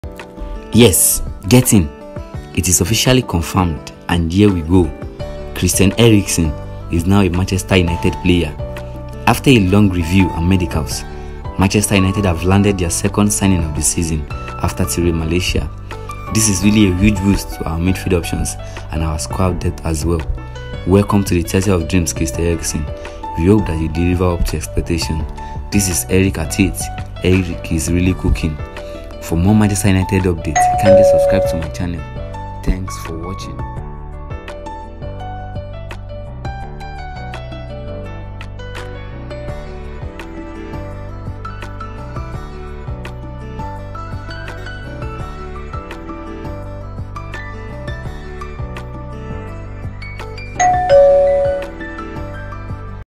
✅Get in!! Christian Erikson is now a red🔴. Erik ten Hag makes his second summer signing. 😀❤️✌🏻
Manchester makes their second summer signing. Erikson is now a United player.🔴❤️

Musician: @iksonmusic